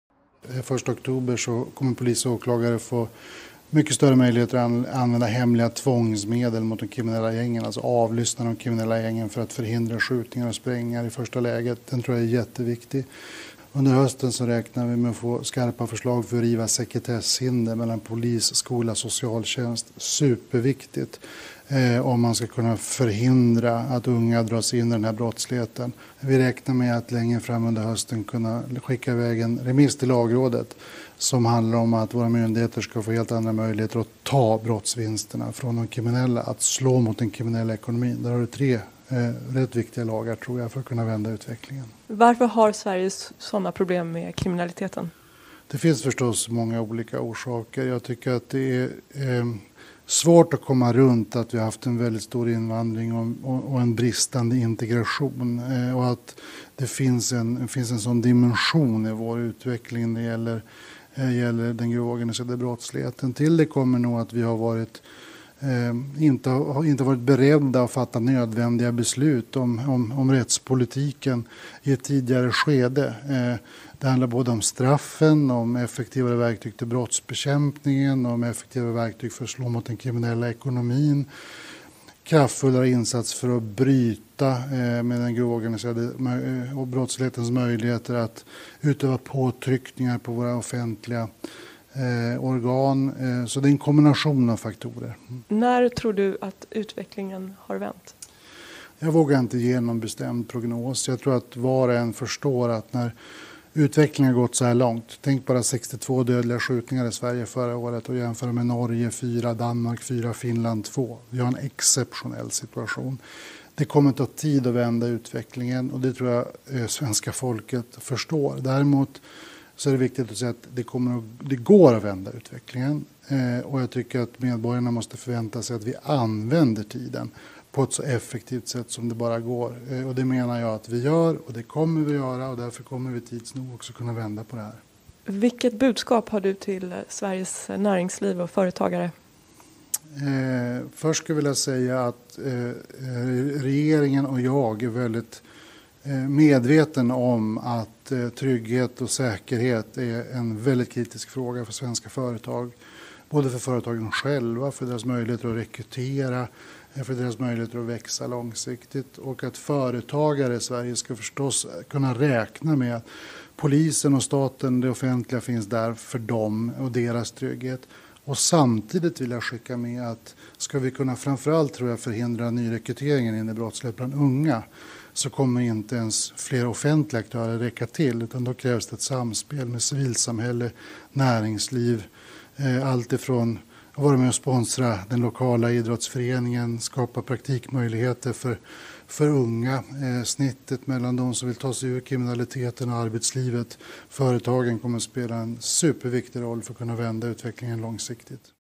Strömmer: Räkna med att det kan bli värre innan det blir bättre
”Räkna med att det kan bli värre innan det kan bli bättre”, säger justitieminister Gunnar Strömmer.